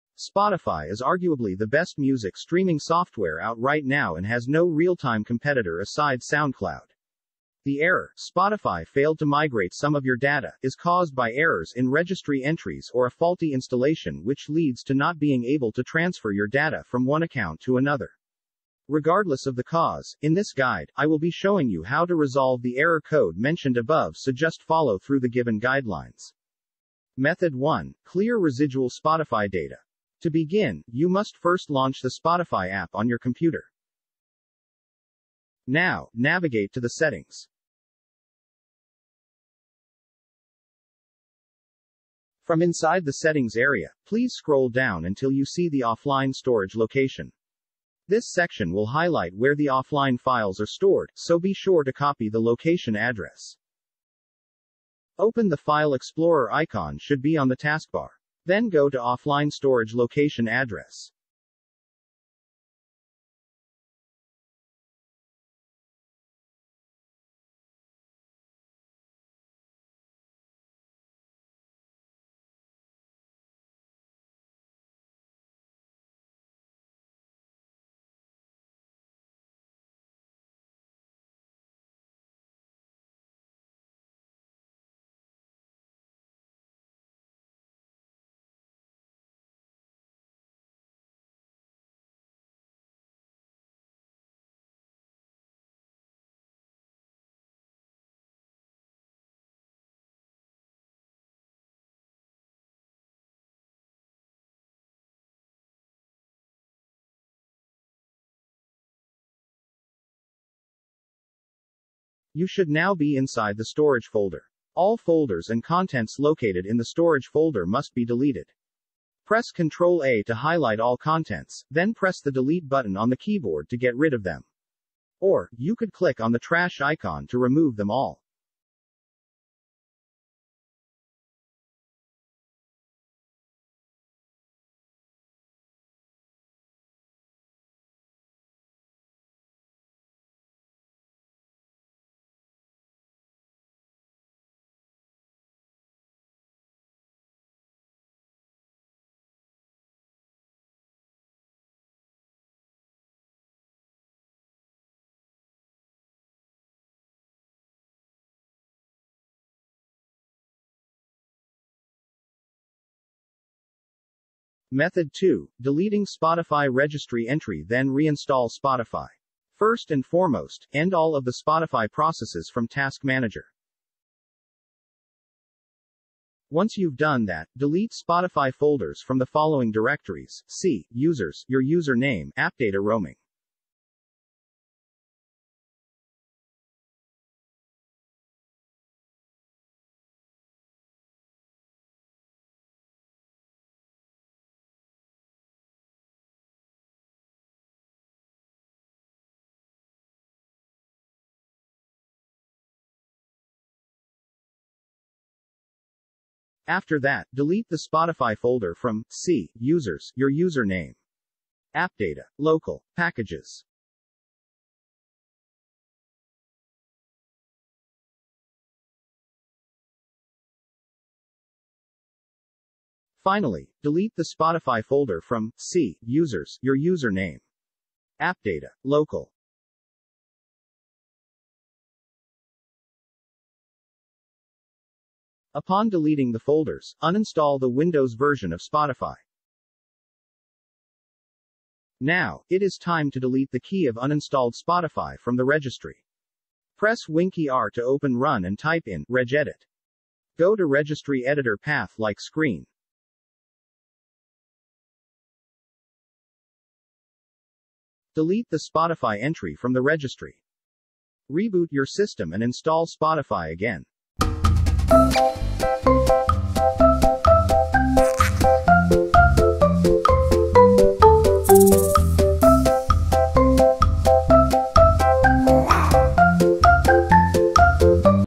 2 Ways To Fix Spotify Failed to Migrate Some of your Data
Spotify is arguably the best music streaming software out right now and has no real-time competitor aside SoundCloud. The error ‘Spotify failed to migrate some of your data’ is caused by errors in registry entries or a faulty installation which leads to not being able to transfer your data from one account to another.

Regardless of the cause, in this guide, i will be showing you how to resolve the error code mentioned above so just follow through the given guidelines.